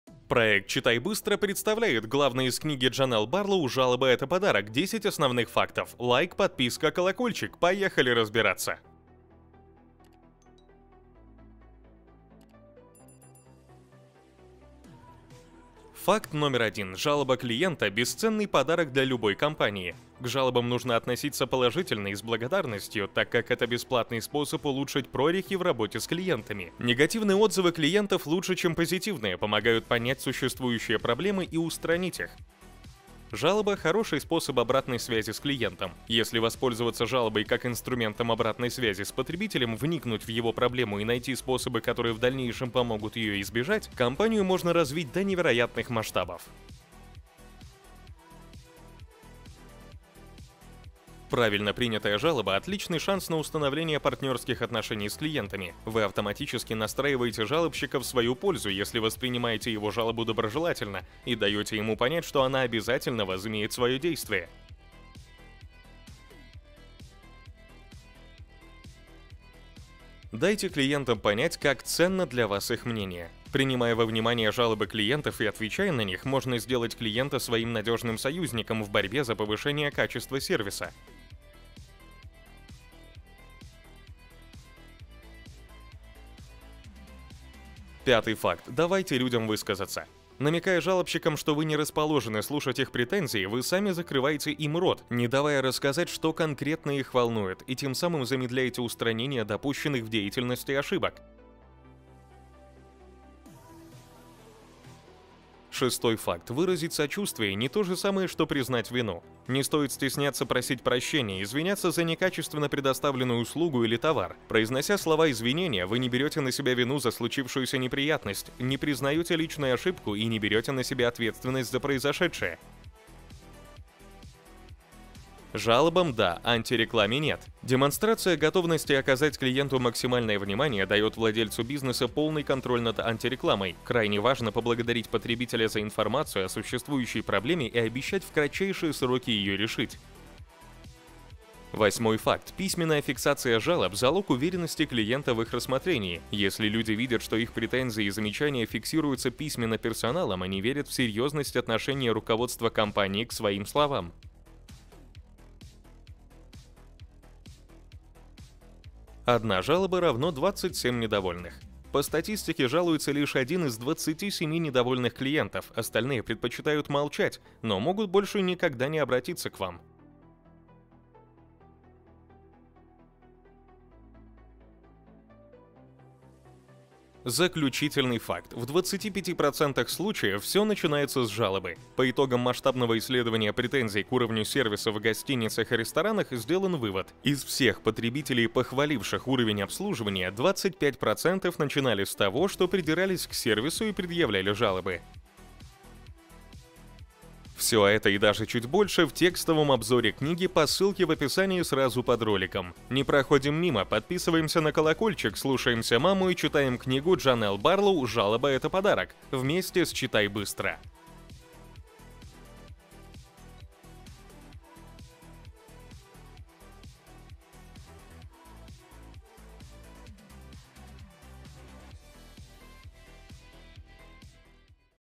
«Жалоба - это подарок» - Книга очень кратко за 3 минуты. Быстрый обзор ⏰
👋 10 фактов из книги Джанелл Барлоу «Жалоба - это подарок» всего за 3 минуты. Тут вот весь обзор целиком

🖤 Если было полезно: лайк, подп... ай, лучше книгу хорошую почитайте и все :)

📚 Читайте книги, слушайтесь маму!